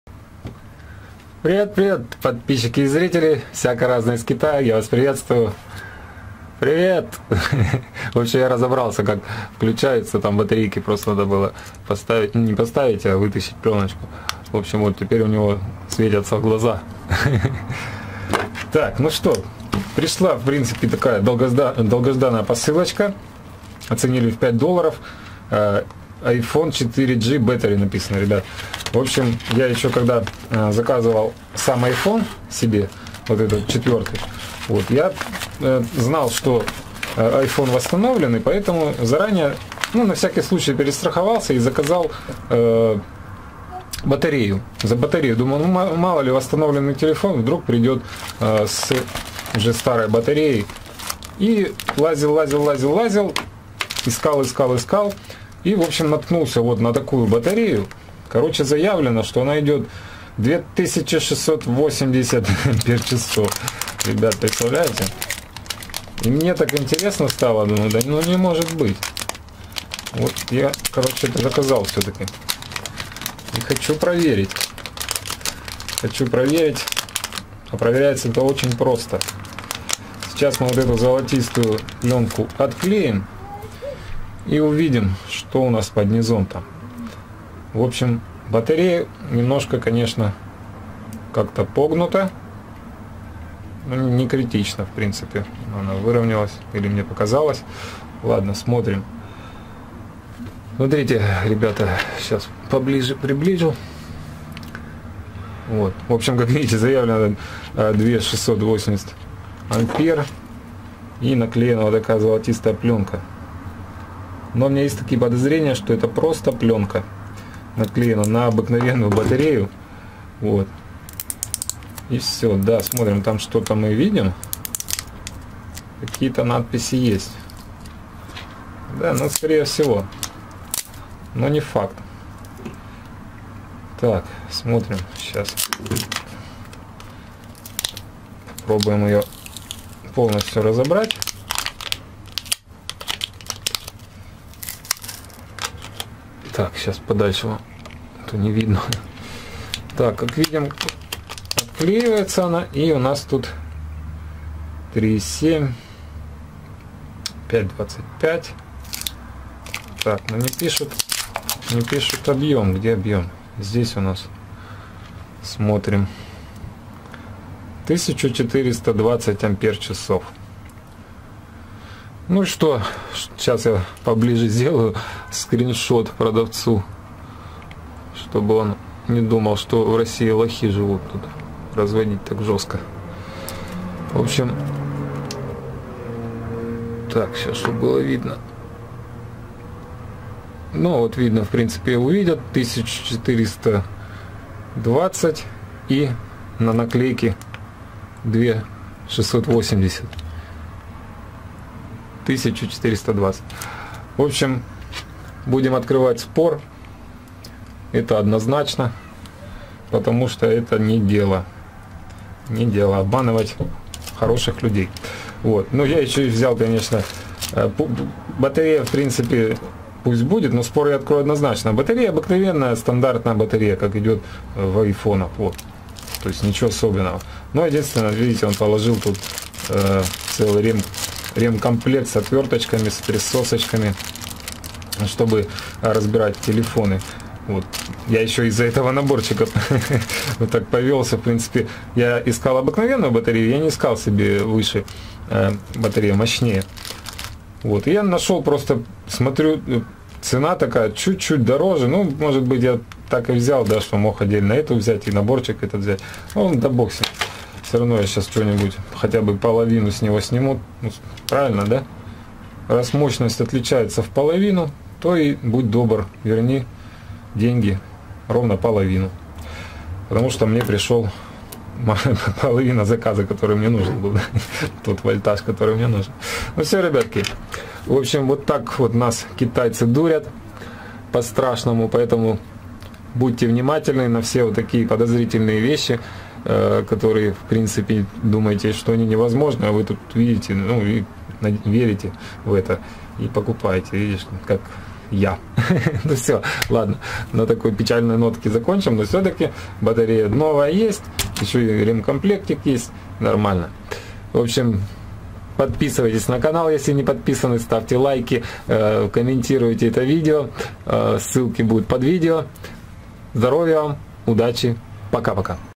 Как нас обманывают китайские продавцы.Посылка Поддельна батарея для айфон 4 с Алиэкспресс
Зарабатывайте в интернете, рекомендуя Вашим пользователям качественные товары с одной из лучших торговых площадок в мире!
● НЕ ЗАБЫВАЙТЕ ПОДПИСЫВАТЬСЯ И СТАВИТЬ LIKE!